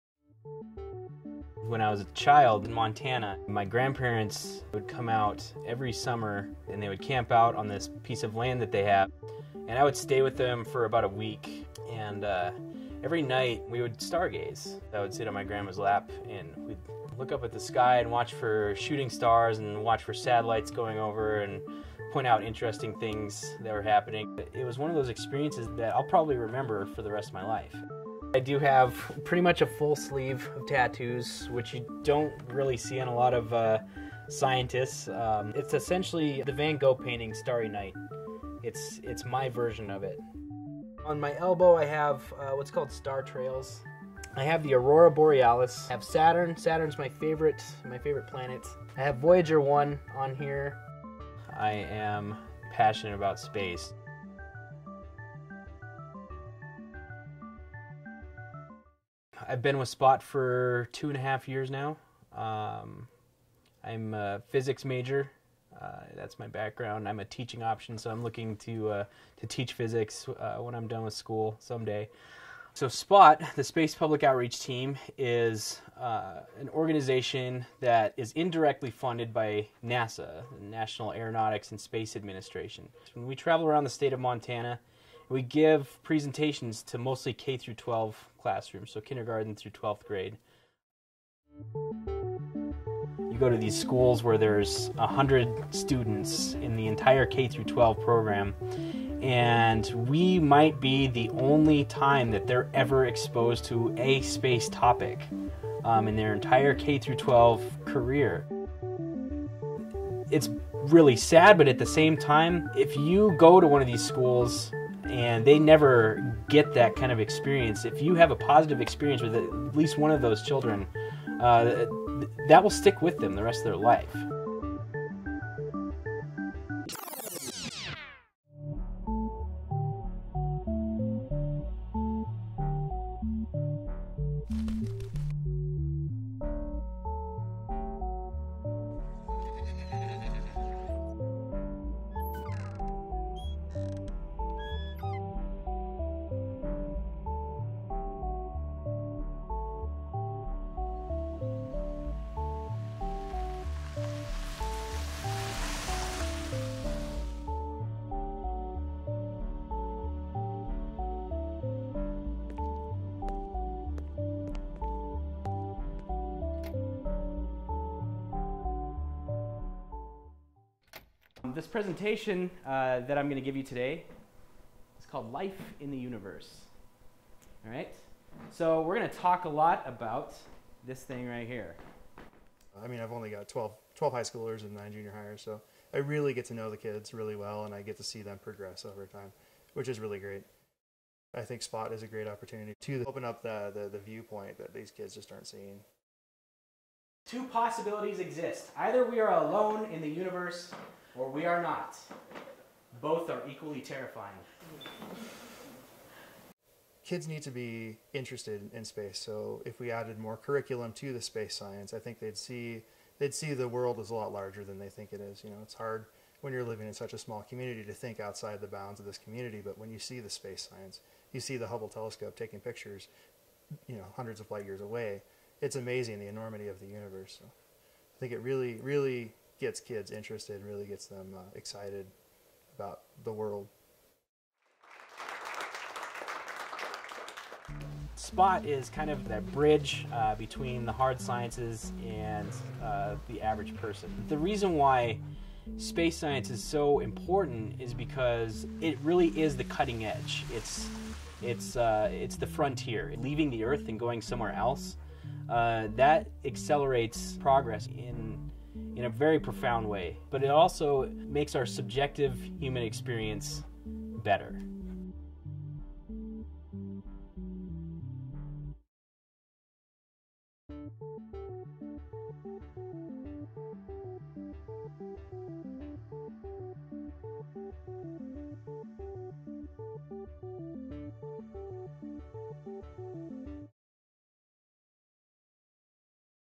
A Day in The Life of a SPOT Ambassador
A film about Montana Space Grant Consortium's Space Public Outreach Team and how it's inspiring kids around Big Sky Country. By SPOT manager Brett Kuxhausen